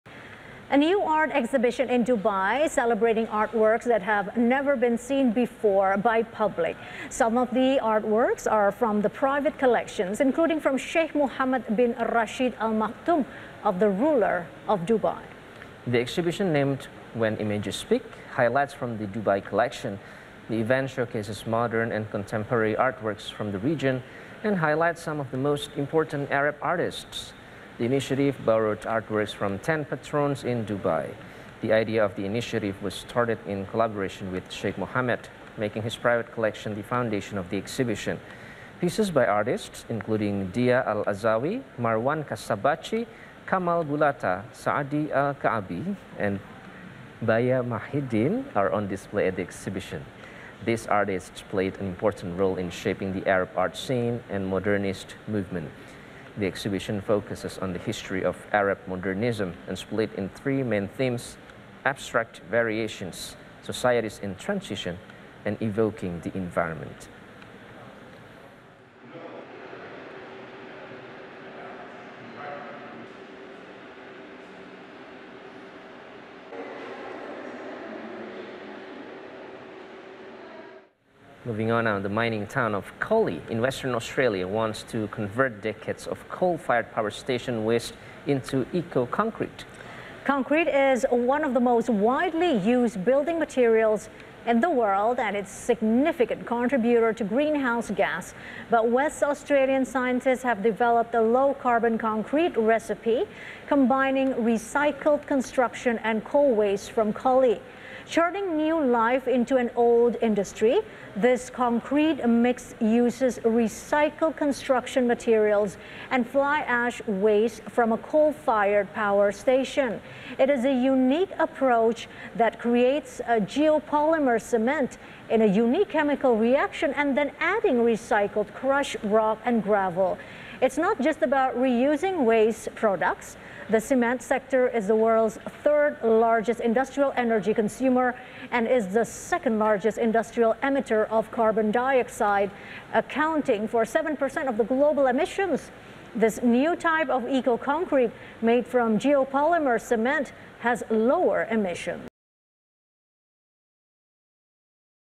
Art Exhibition Reflects Arabian New Art
A new art exhibition holds in Dubai to celebrating art works that have never been seen before by public. Some of the artworks are from the private collections, including from Sheikh Mohammed bin Rashid Al-Maktoum the ruler of Dubai. Meanwhile, the mining town of collie in Western Australia wants to convert decades of coal fired power station waste into eco concrete.